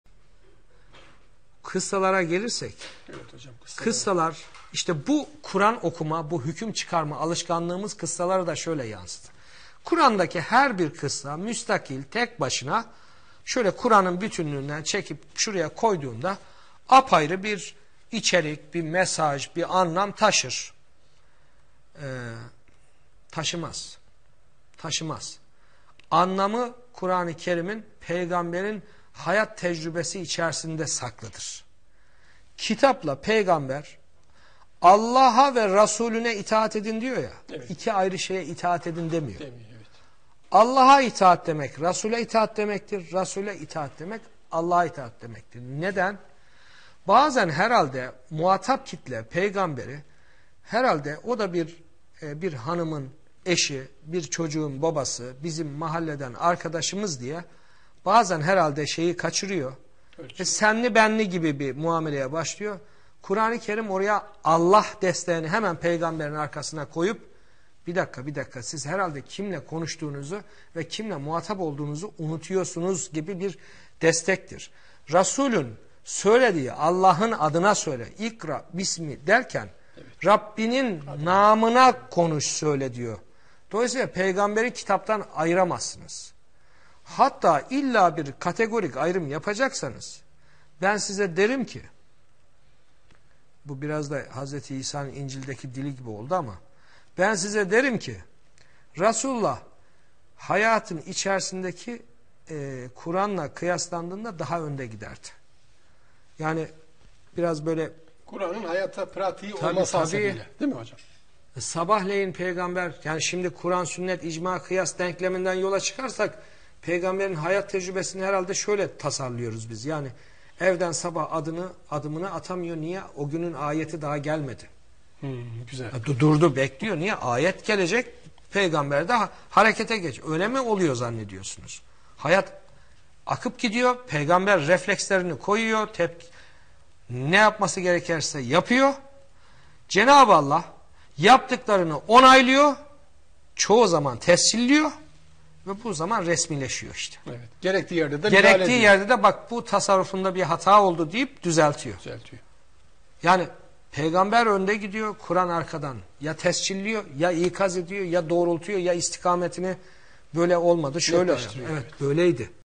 Peygamber Evden Çıkmak İçin Ayet Gelmesini Beklemedi - Mustafa Öztürk
Kıssalar, Kur'an'ın bütünlüğünden çekilip çıkarıldığında tek başına bir anlam taşımaz. Anlamı, Hz. Peygamber'in hayat tecrübesi ile birleştirilince ortaya çıkar.

Ayet "Allah'a ve resulüne itaat edin" derken iki ayrı şeye itaat etmemizi söylemiyor. Allah'a itaat resule itaat demektir, resule itaat Allah'a itaat demektir. Bu ayet muhtemelen muhatap kitlenin Hz. Peygamber'i "o da bir hanımın eşi, bir çocuğun babası, bizim mahalleden arkadaşımız" olarak görüp ölçüyü kaçırması ve senli-benli bir muameleye başlaması durumunda Kur'an hemen oraya Allah desteğini koyup "bir dakika, siz kiminle muhatap olduğunuzu unutuyorsunuz" gibi bir uyarı yapması için geliyor.

Resul zaten Allah'ın adına konuşur. Başka bir ayette "ikra bismi" derken "rabbinin namına söyle" diyor. Dolayısıyla peygamberi kitaptan ayıramazsınız.

Hatta kategorik kıyas yaptığınızda "Kur'an, sünnet, kıyas, icma" sırası farklılaşır. Biz bu mevcut sıranın oluşturduğu "Kur'an söyler, peygamber uygular" denklemine göre baktığımızda sanki Hz. Peygamber'in hayat tecrübesinin şöyle olduğunu zannediyoruz: Hz. Peygamber sabah evden çıkacak ama dışarı adımını atamıyor. Neden?  O günün evden çıkma ayeti henüz gelmedi. Durup ayeti bekliyor.

Oysa böyle olmuyordu. Hayat olanca hızıyla akıp giderken Hz. Peygamber reflekslerini ortaya koyuyor, ne yapması gerekirse yapıyor; Kur'an onu çoğu zaman tescilliyor, gerektiği yerde de "şu tasarrufunda bir hata oldu" diyerek düzeltiyor.

Kur'an'la Yüzleşme - 4 Haziran 2016

*Yukarıdaki hesaplar Prof. Dr. Mustafa Öztürk'ün bilgisi dahilinde açılmıştır ancak bizzat kendisi tarafından yönetilmemektedir.